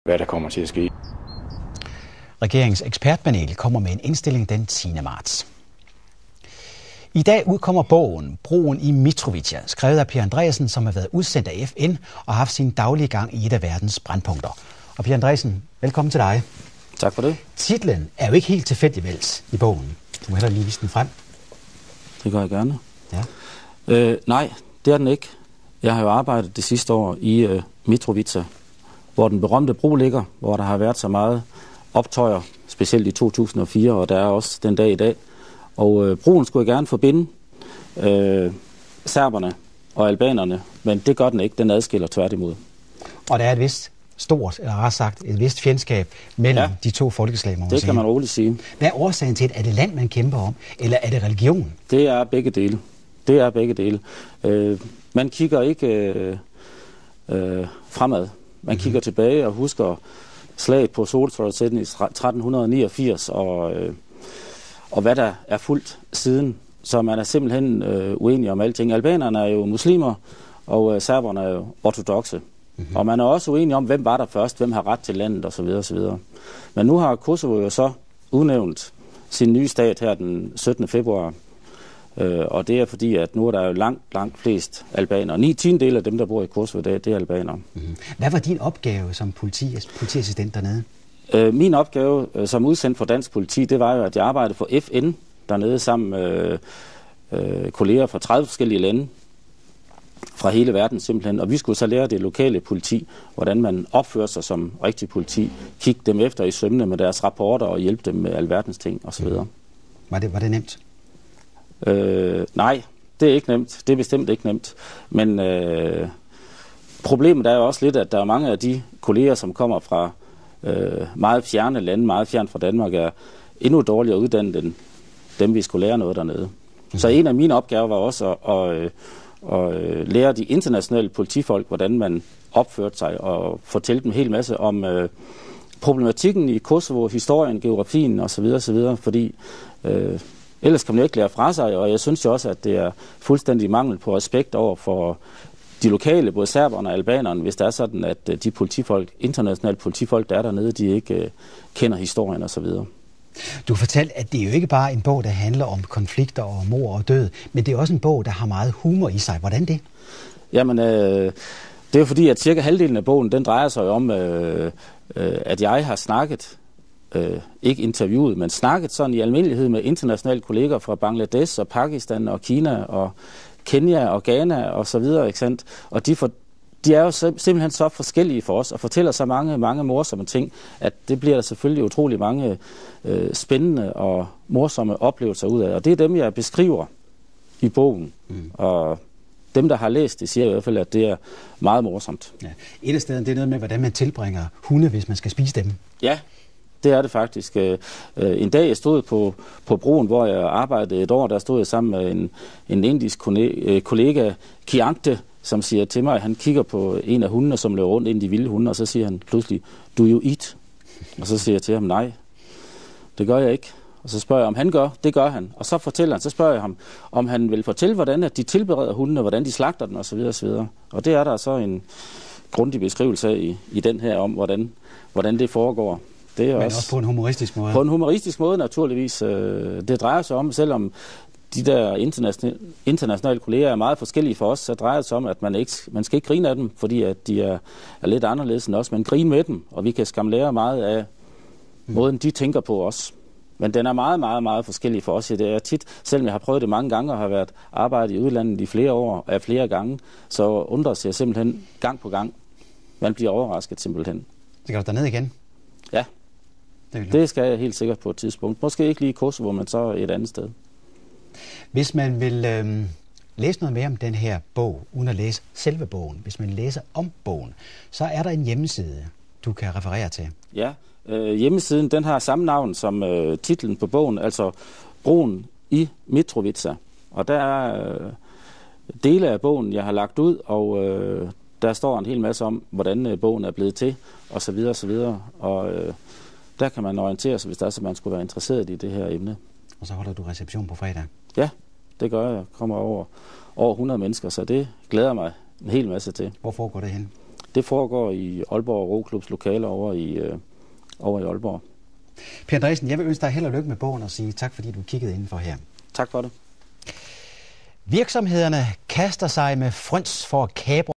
Broen I Mitrovica - TV2 Nord
Per Andreasen er netop udkommet med bogen "Broen i Mitrovica". Se klip fra TV2 Nord.